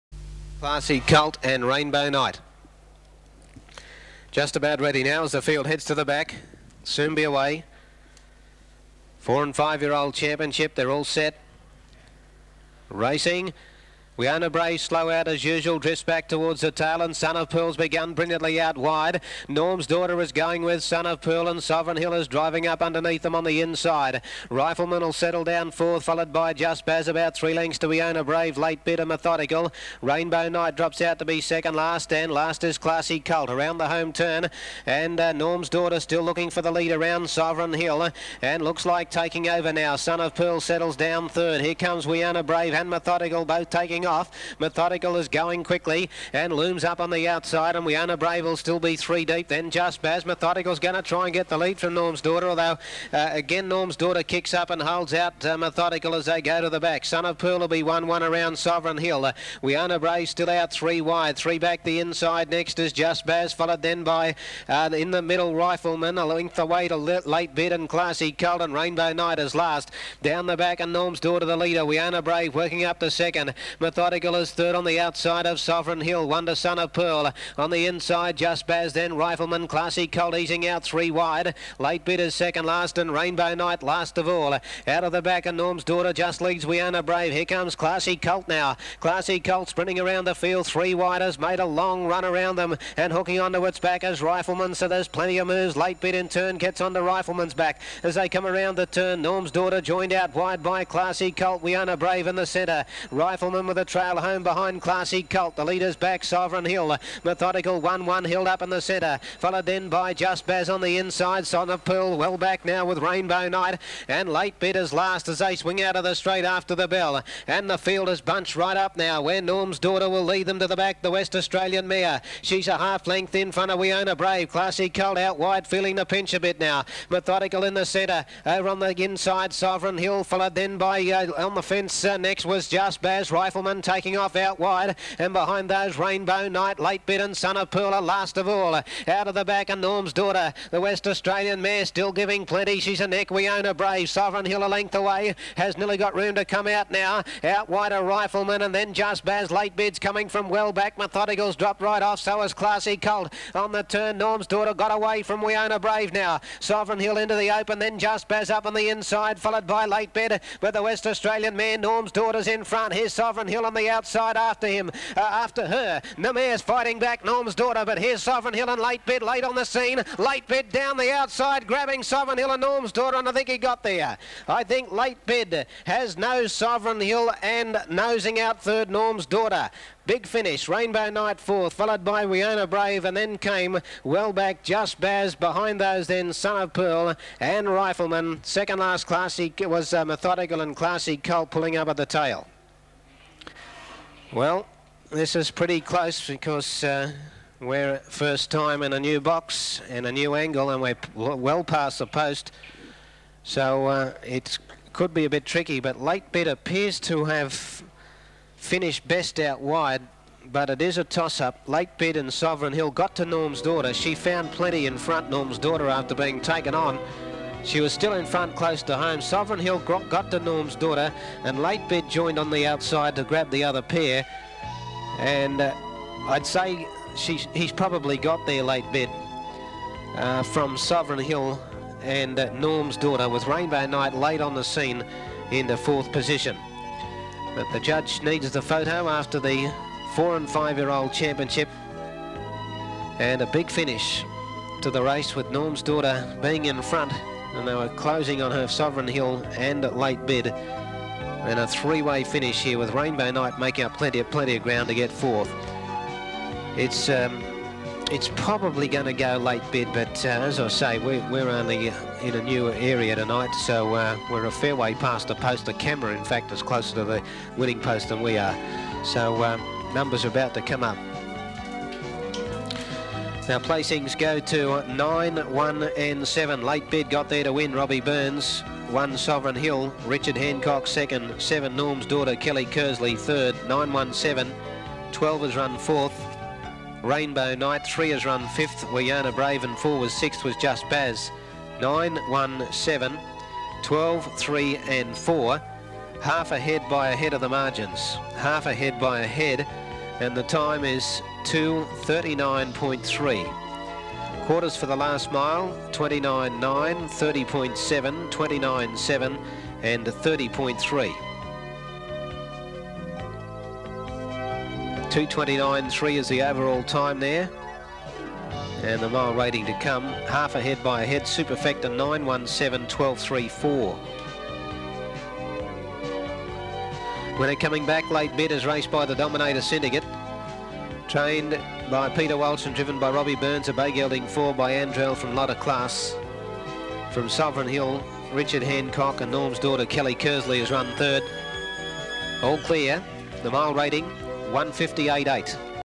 Late Bid - 1996 4 and 5yo Championship
Late Bid wins the 1996 Four and Five-year-old Championship at Harold Park.